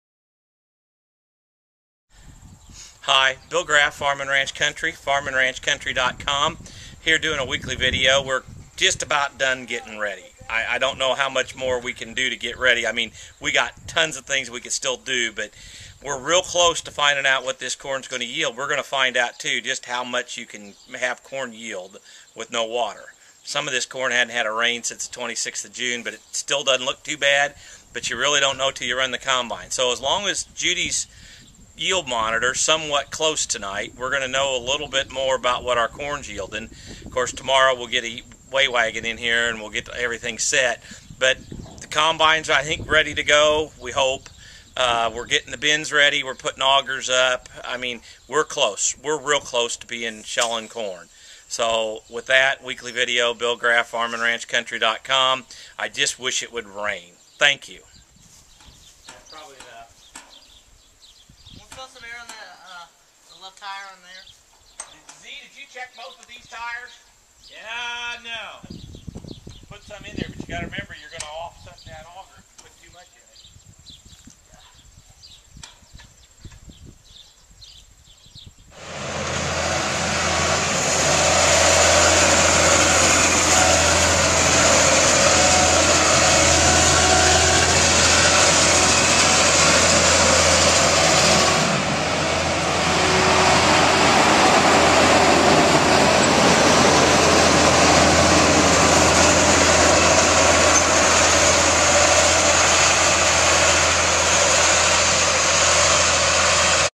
Getting Ready For Harvest
We'll see how the corn yields with very little rain. Combine is ready. Setting up the auger to the grain bin.